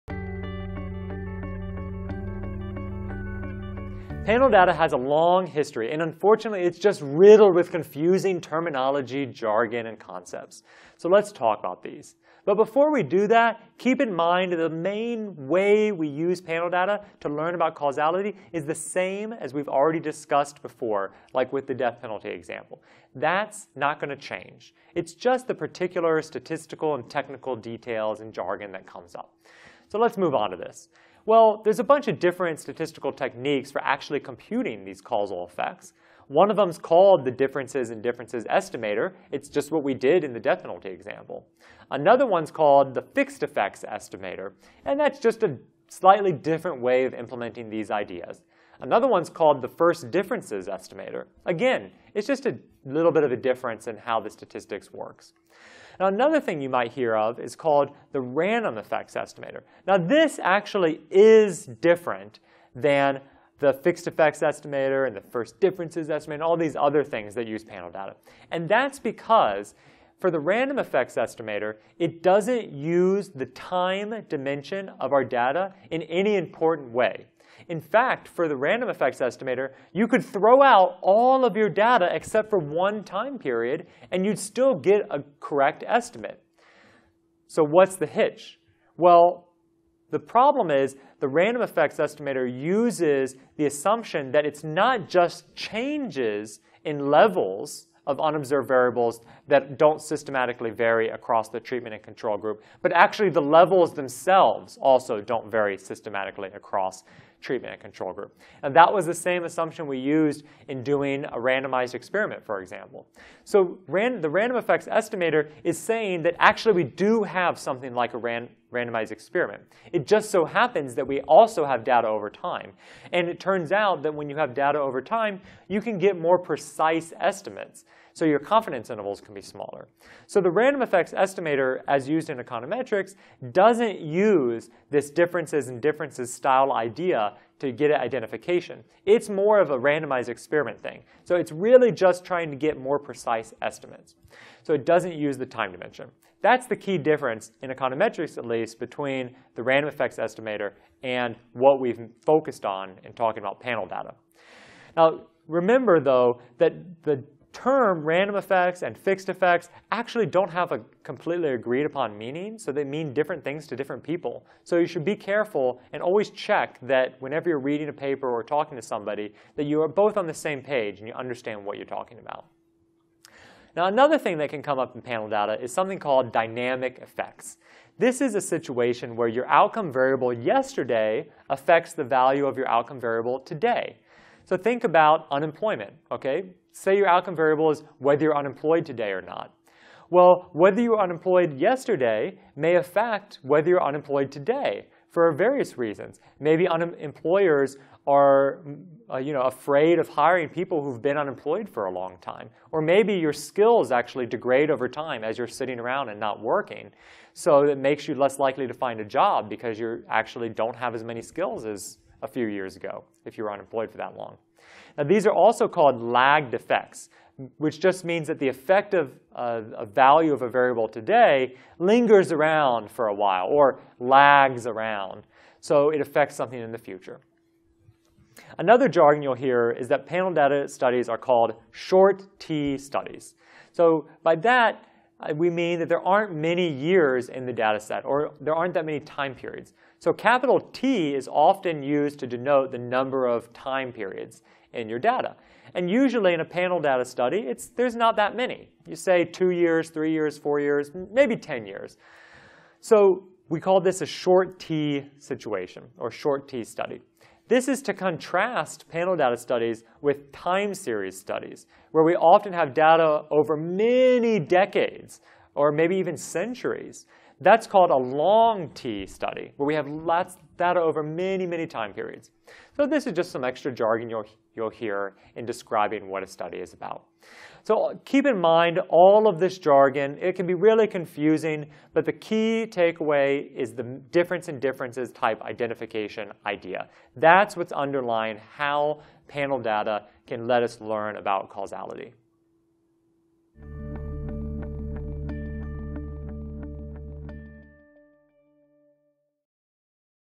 Panel Data Terminology: Causal Inference Bootcamp
There are some technical terms that are often used when analyzing panel data, so we give you a quick rundown of the major keywords to look for.